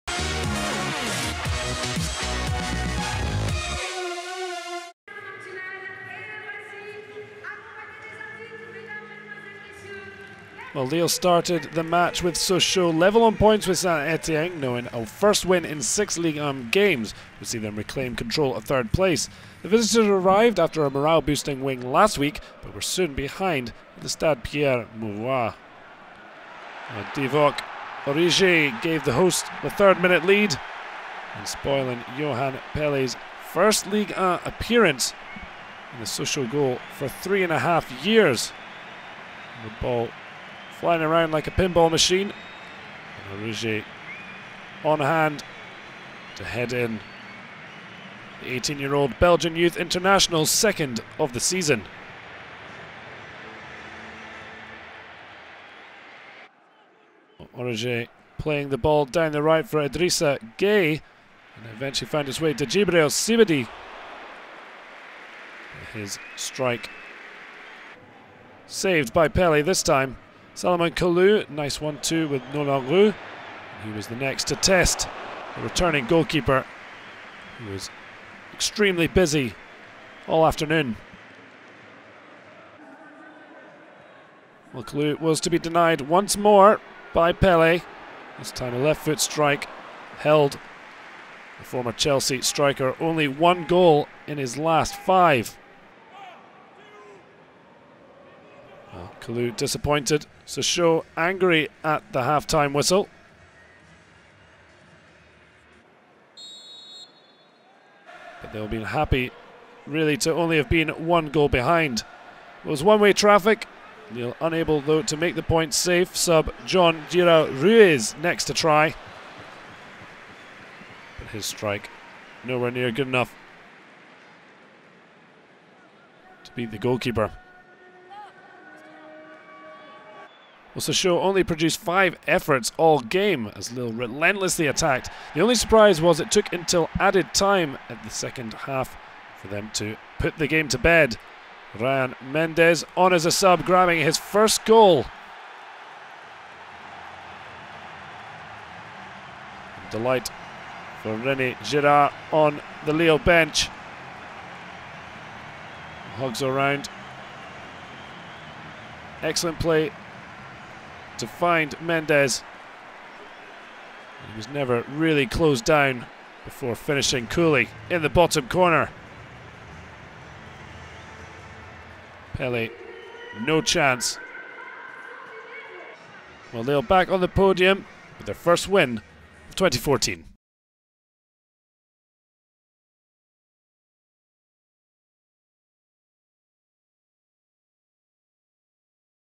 LOSC Lille - FC Sochaux-Montbéliard (2-0) - 08/02/14 - (LOSC-FCSM) -Highlights
LOSC Lille vs FC Sochaux-Montbéliard (2 - 0) highlights. The best actions and goals of LOSC Lille vs FC Sochaux-Montbéliard in video.

Stade Pierre Mauroy - Sat, 08 Feb 2014

Goals : Divock ORIGI (3' - LOSC Lille) / Ryan MENDES (90' +3 - LOSC Lille)

LOSC Lille Team line-up : 1 - Vincent ENYEAMA / 4 - Florent BALMONT / 5 - Idrissa GUEYE / 8 - Salomon KALOU / 14 - Simon KJAER / 15 - Djibril SIDIBE / 23 - Pape SOUARE / 24 - Antonio MAVUBA (c) / 25 - Marko BASA / 26 - Nolan ROUX / 27 - Divock ORIGI